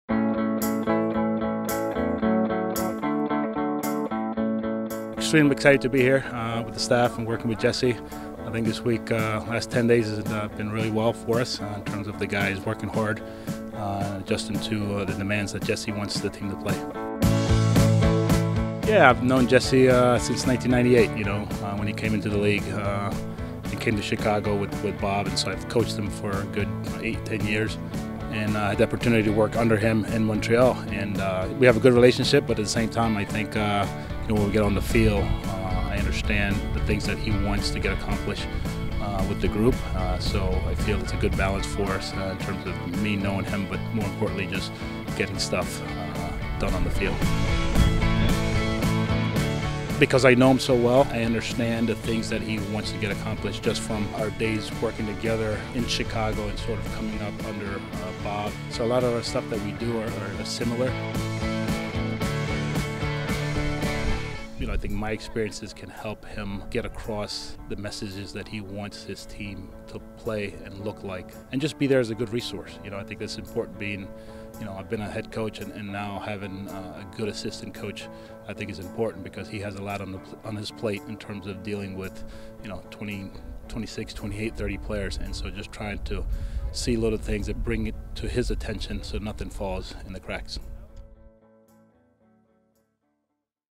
New York Red Bulls - Meet Denis Hamlett
New assistant coach Denis Hamlett talks about joining the New York Red Bulls and his relationship with head coach Jesse Marsch.